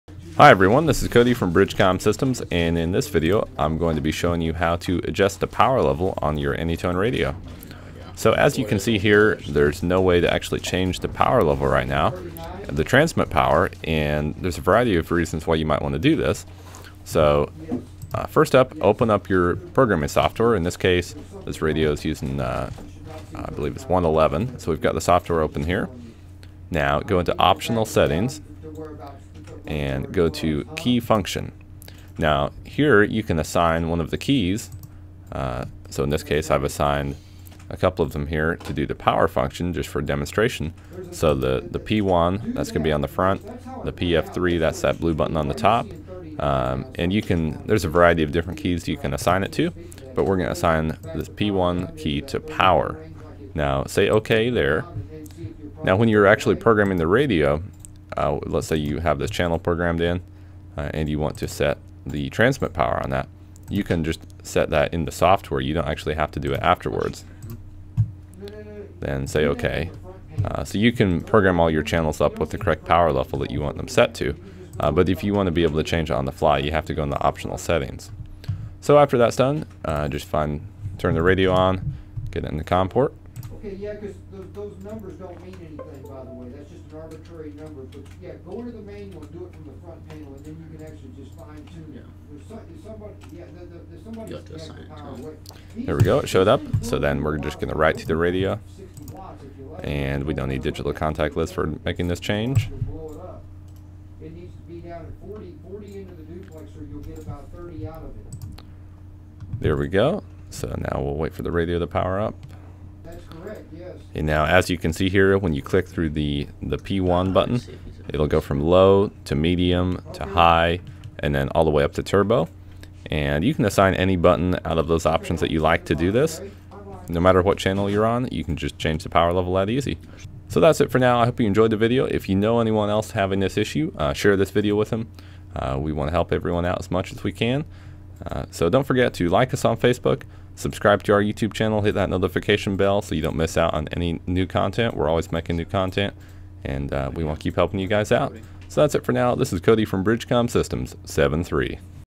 How to Change the Transmit Power of the AnyTone HT
Transcription:
Hi, everyone. This is Cody from BridgeCom Systems. In this video, I'm going to be showing you how to adjust the power level on your AnyTone radio. As you can see here, there's no way to actually change the power level right now, of the transit power. There's a variety of reasons why you might want to do this. First up, open up your programming software, in this case, this radio is using, I believe, it's 1.11. We've got the software open here. Now go into optional settings and go to key function.
Now here you can assign one of the keys, so in this case I've assigned a couple of them here to do the power function just for demonstration, so the P1. That's going to be on the front. The PF3, that's that blue button on the top. There's a variety of different keys you can assign it to, but we're going to assign the P1 key to power. Now say okay there. Now when you're actually programming the radio, let's say you have this channel programmed in, and you want to set the transmit power on that, you can just set that in the software. You don't actually have to do it afterwards. Then say, okay. You can program all your channels up with correct power level that you want them set to, but if you want to be able to change on the fly you have to go in the optional settings.
After that's done, just turn the radio on, get in the com port. There we go, it showed up. Then we're just going right to the radio, and [inaudible] digital contact list for making this change. There we go, so now we'll wait for the radio to power up. Now as you can see here when you click through the P1 button it'll go from low, to medium, to high, and then all the way up to turbo. You can assign any button out of those options that you like to do this. No matter what channel you're on you can just change the power level that easy.
That's it for now. I hope you enjoyed the video. We want to help everyone out as much as we can. We're always making new content, and we want to keep helping you guys out. That's it for now. This is Cody from BridgeCom Systems 73.
END TRANSCRIPTION

Or just go straight to our website and browse our catalog.